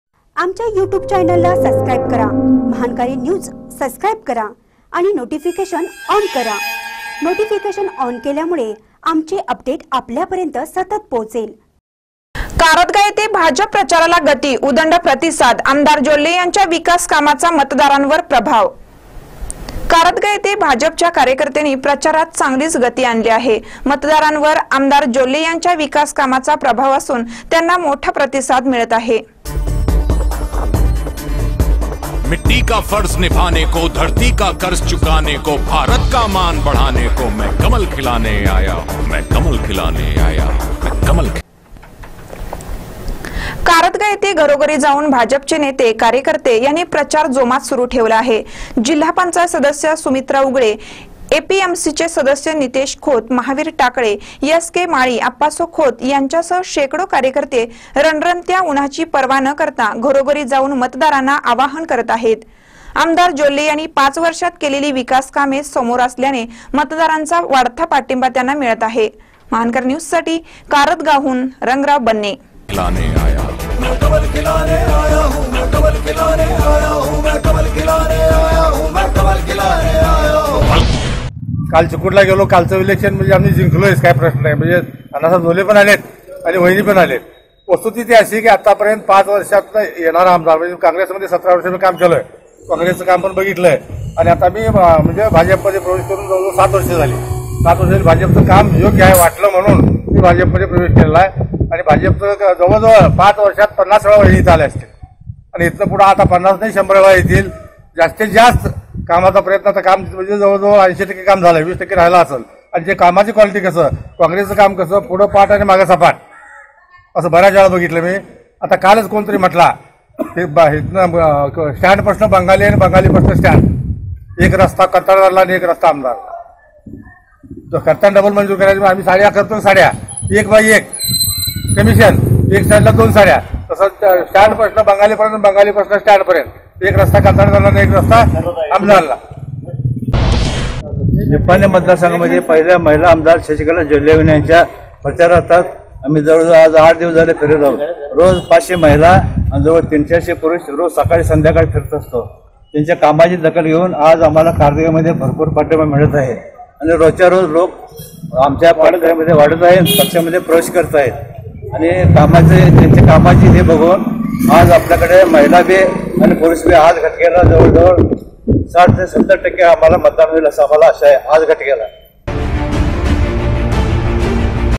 कारदगा येथे भाजप प्रचाराला गती, उदंड प्रतिसाद
कारदगा येथे भाजप प्रचाराला गती, उदंड प्रतिसाद : आ.जोल्ले यांच्या विकास कामांचा मतदारांवर प्रभाव